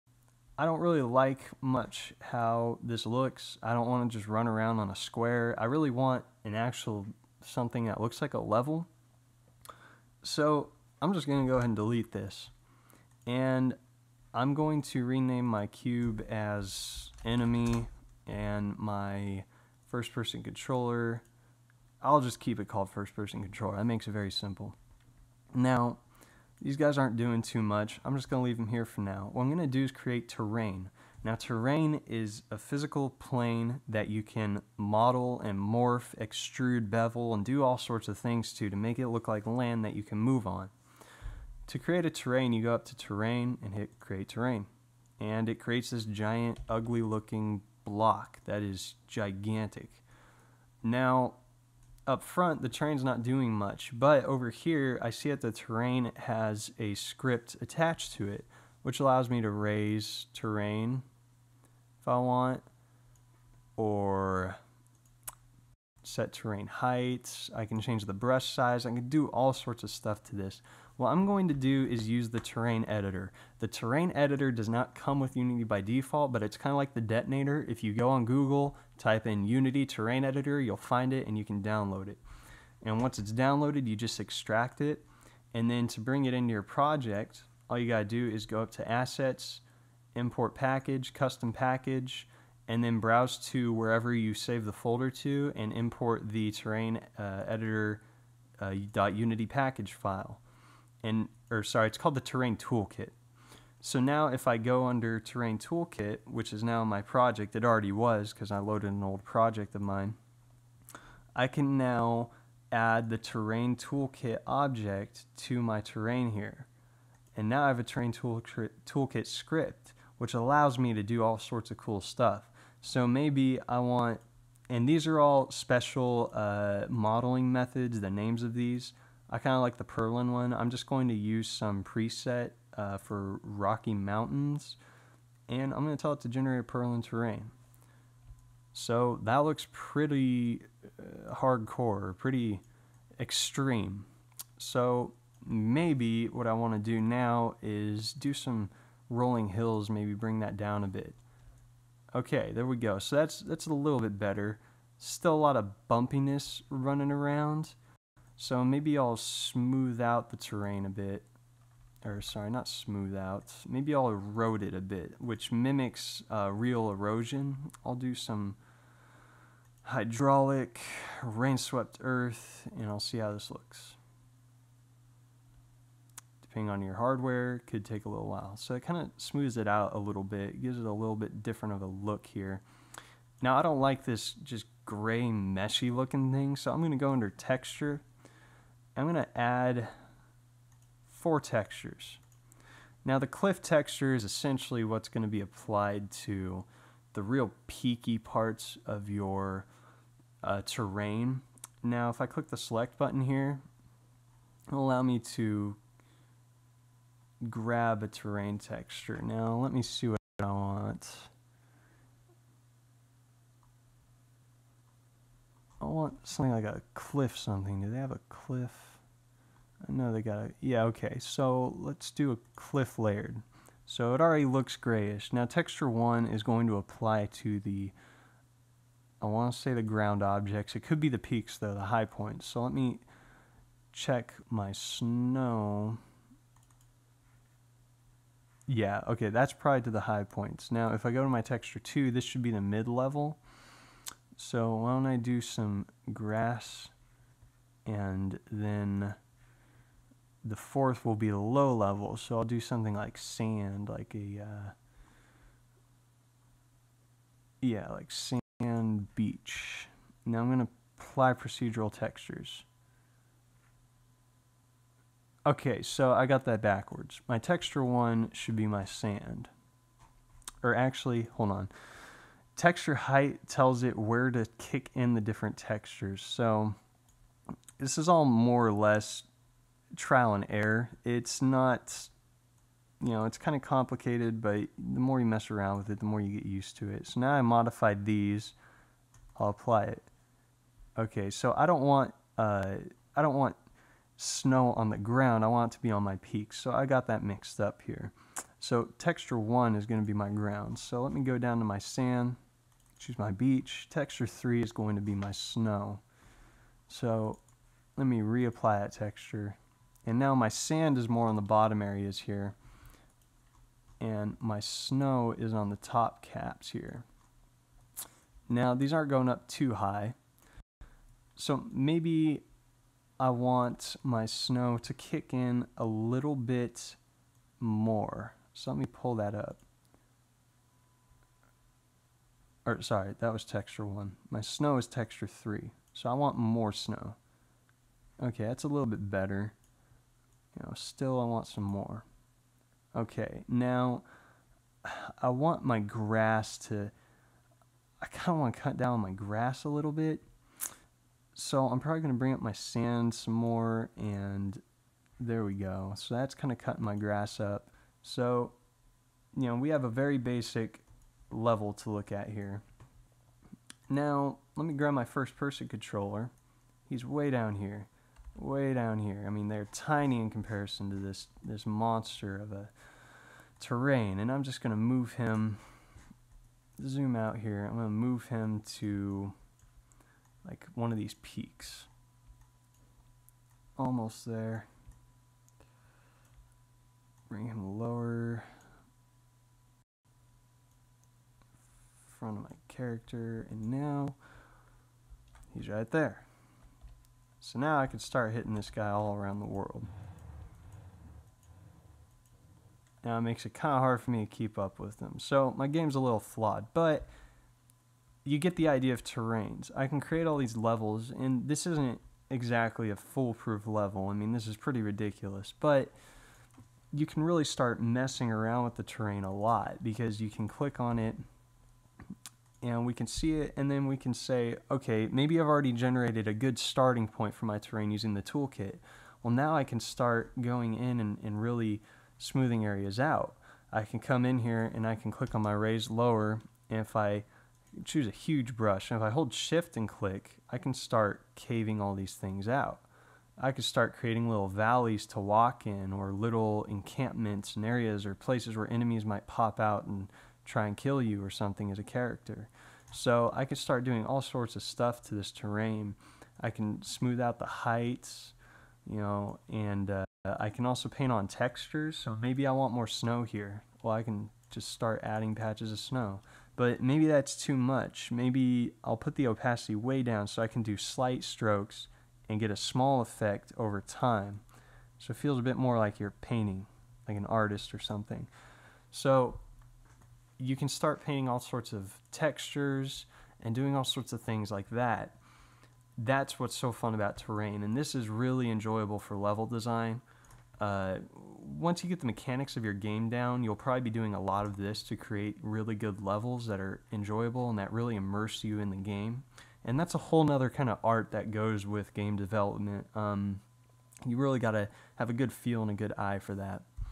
Unity 3D Tutorial Part 4 Terrain
Learn how to create realistic levels for your games using terrain. This short video series provides a look at the absolute basics of using the Unity game Engine with Unity 3D to develop video games. Students can get a large discount on Unity 3D Pro if you can provide proof of your status as a student.

Looking for more detailed tutorial videos? Try design3